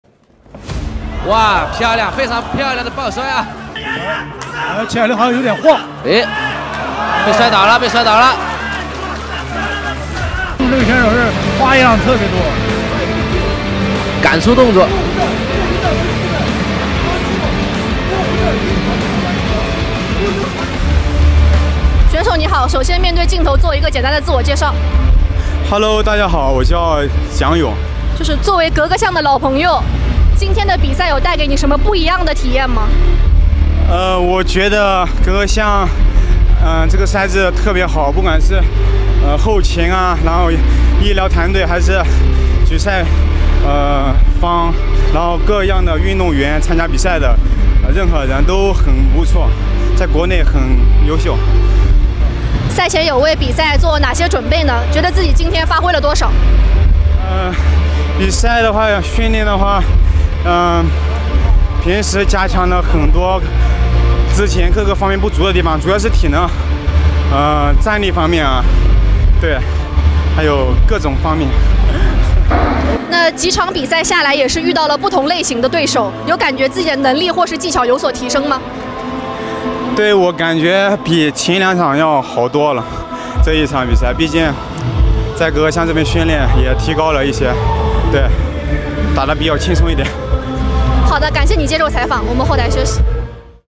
Yong Jiang's interview ｜satisfying fight at Happy Elephant MMA Champions League
In this exclusive interview, we delve into Yong Jiang's rigorous training at Happy Elephant MMA and witness his exceptional performance during the competition. From his unwavering dedication to his exceptional skills, Yong Jiang showcases a level of mastery that leaves the audience in awe. Join us as we relive the moments of his triumphant victory and discover the secrets behind his success. Don't miss out on this thrilling display of athleticism and determination!